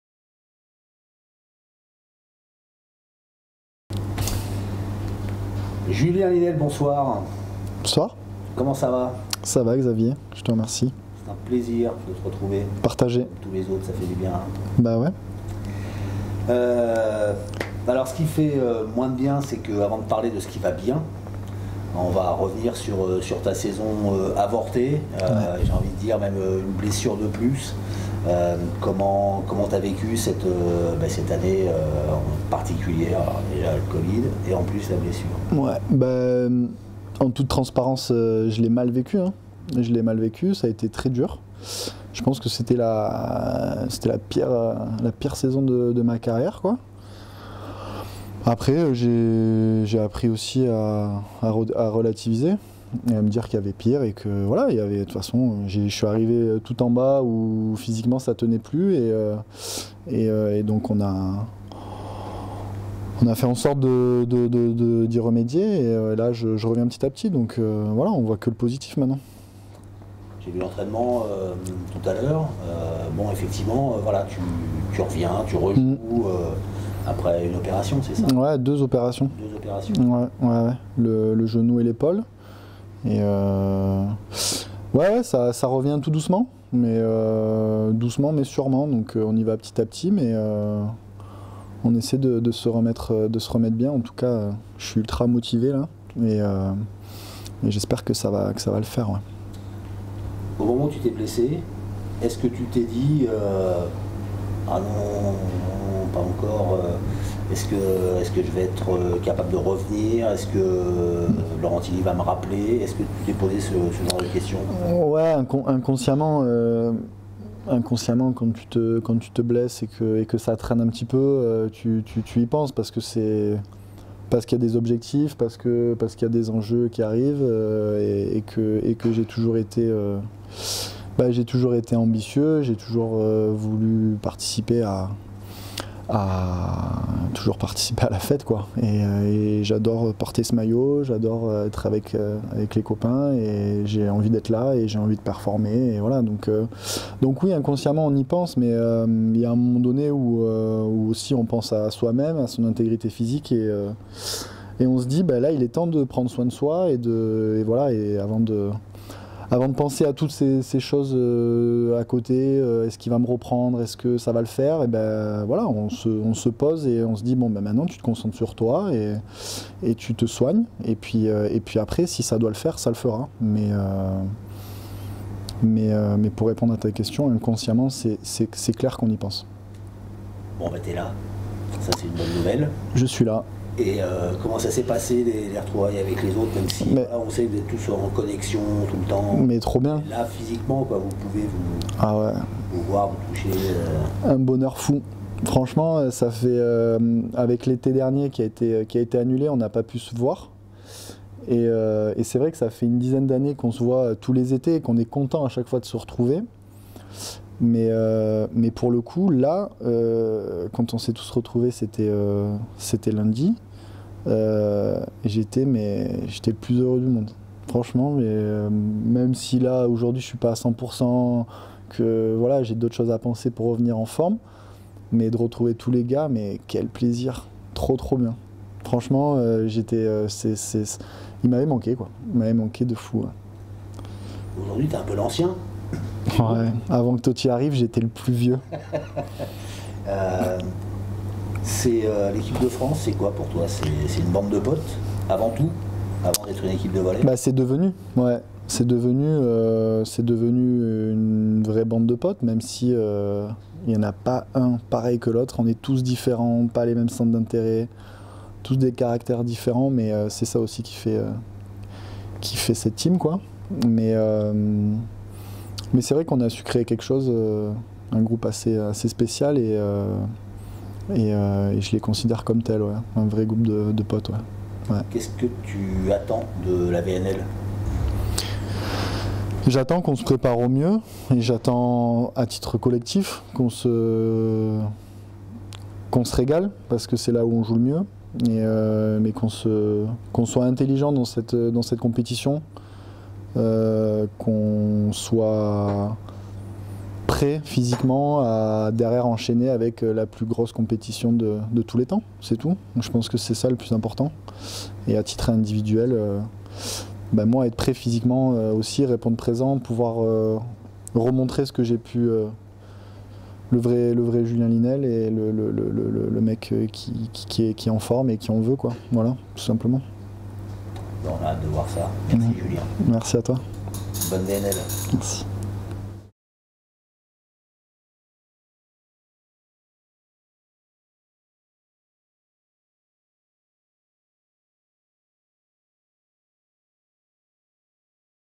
Julien LYNEEL - Préparation VNL 2021
Après une saison difficile marquée par les blessures et les opérations Julien Lyneel savoure de retrouver l'Equipe de France et ses potes. Il nous raconte